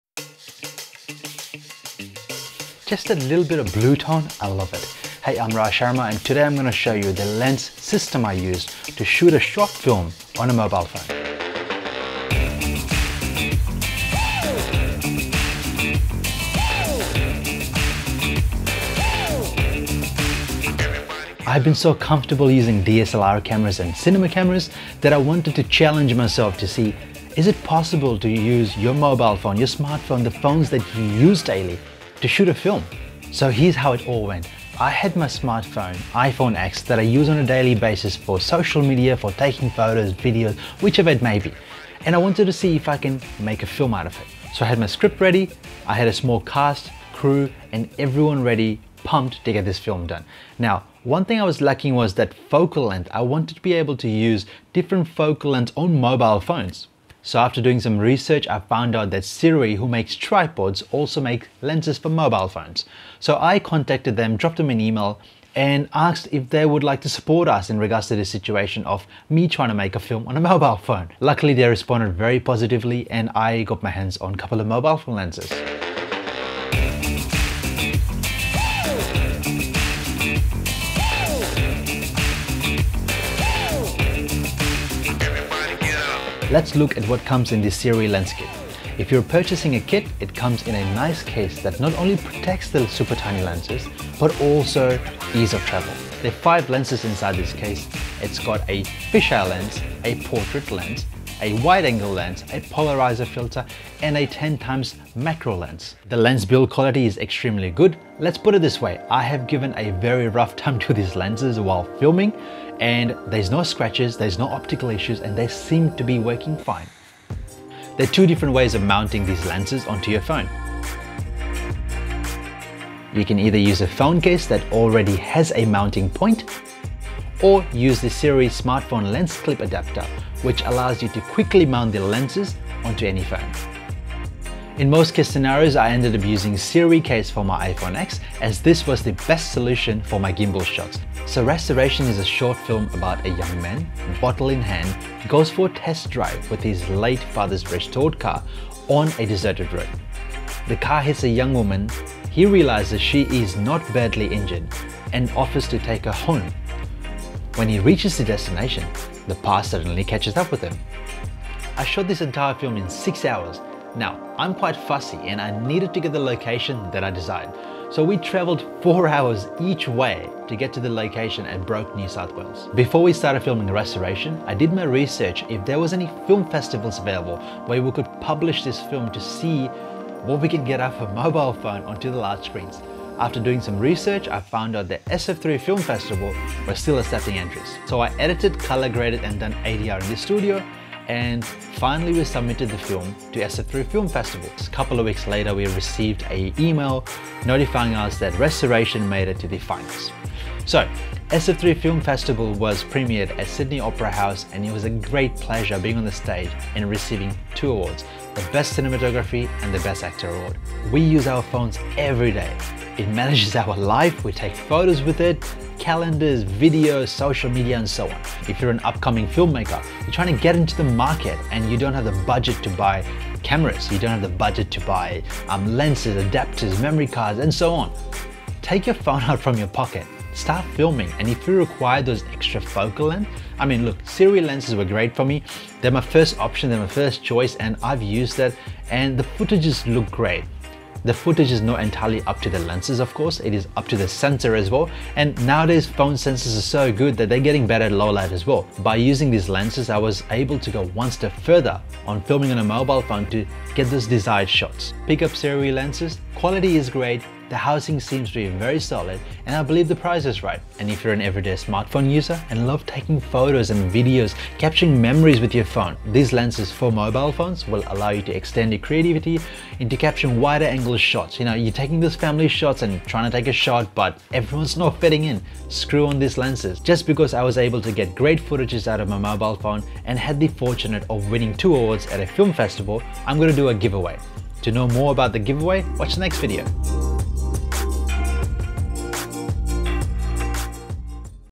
Sirui Lens Review - Smartphone Lens - Rah Sharma
Hey I am Rah Sharma and today I am going to show you the lens system I used to film a short on a mobile phone!

I have been so comfortable using dslr and cinema cameras that I wanted to challenge myself to film on a mobile phone. So here’s how it all went..

I had my phone that I use everyday, iPhone X, a story, super small cast and crew and that one thing was missing. Focus length - how to get those different shots using smartphone?

After doing some research I approached Sirui Australia, glad to see that Sirui responded and was happy to supply set of lens for my iPhone for my short film.

Let’s look at what comes with Sirui lens kit. If you’re purchasing a kit, it comes in this nice case that not only protects the super tiny phone lens but also for ease of travel.

There are 5 lens inside this case
170 Degrees Fisheye Lens
18mm Wide Angle
60mm Portrait Lens
Macro 10x Lens
And a polariser filter

These lenses build quality are extremely good, let me say I have given them a rough time and they still seem to have no scratch and optical issues!!

There are two different ways of mounting these lens onto your smart phone. You can either get a phone case that already has a mounting point build in or use Sirui Smartphone Lens Clip which is very quick to mount lens to any phone!

In most of the scenarios I ended up using Sirui case for my iPhone X as this was the best solution for my gimbal shots. After all 90% of the shots in Restoration, the short film I shot with these lens were on gimbal.

So getting back to the film, “Restoration” is a short film about a young man, bottle in hand, goes for a test drive with his late father's restored car on a deserted road. The car hits a young woman, he realises she is not badly injured and offers to take her back home. When he reaches his destination, the past suddenly catches up with him.

I shot this entire film within 6 hrs. Let me say this, I am very fussy, I desired a location and that had to be it. We travelled almost 4 hrs each way for to this location in Broke, NSW. My cast and crew were absolutely supportive and I cannot thank them enough.

It’s scary filming a short film on a mobile phone, nowadays phones shoot 4k videos! It was a good challenge.

Before we started filming “Restoration” I also did my research if there were any film festivals still accepting entries. Someone recommended be SF3 film festival.

I edited, done ADR and color grading, submitted the film to the film festival. A few days later I receive the announcement that we have made it to the finals. I also also scheduled to go to Photokina in Cologne Germany with Zhiyun Tech. Keep an eye out for my vlogging video from Sydney to Shenzhen and Germany.

Had to take a 26 hour flight on the last day of photokina to come back to Sydney as “Restoration” was premiered at SF3 film festival at Sydney Opera House where we were awarded Best Cinematography and Best Actor. So proud that a film shot on a mobile phone made to the large screens.

I was quite surprised that “Restoration” looked really nice on the big screen, thanks to Sirui I was able to get those shots I desired.

If you’re a upcoming filmmaker trying to find ways to crack into the market and don’t have the budget to buy dslr and cinemas cameras, lens etc then look no further then your phone in your pocket, it can be quite handy! I mean it got “Restoration” on the big screens that got us awards.

Pickup these Sirui lens as they are great quality, the housing is solid and I believe the price is just right.

And if you’re just a regular smart phone user wanting to expand your creativity, pickup these lens as they will be perfect for your holiday tips, daily commuting and what so ever!

Now, let me make it more interesting, since I am so happy that I made the most out of my mobile phone, I am going to run a giveaway - give away details coming up in the next video!

Rah Sharma